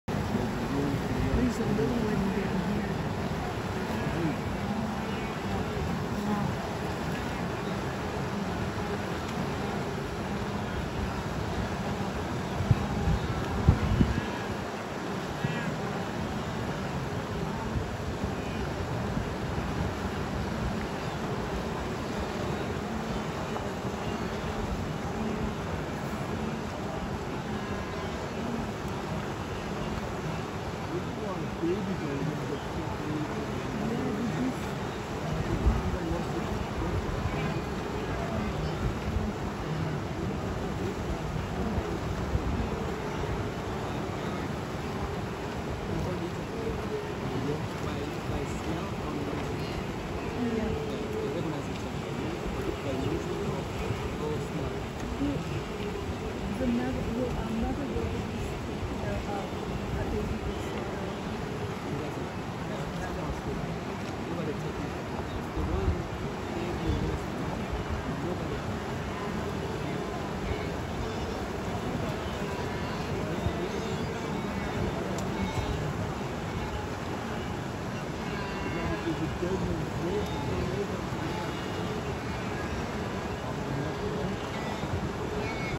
Wildebeest Migration 2024
Here is a little video I made on 9/5/2024 of 1000's of Wildebeest migrating over the Mara River in northern Serengeti National Park in Tanzaina.